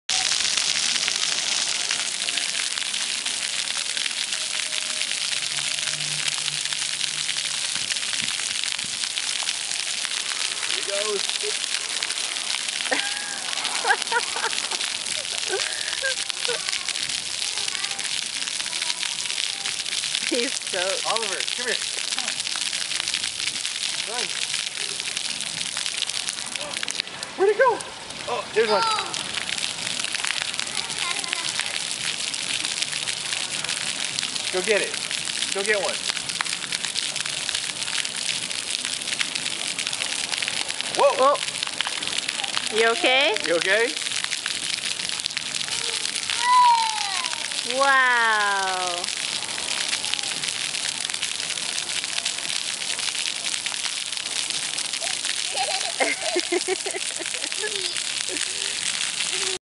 playing in the fountains
went to Riverside, CA for a wedding - it was hot that weekend - We found this fun place to play.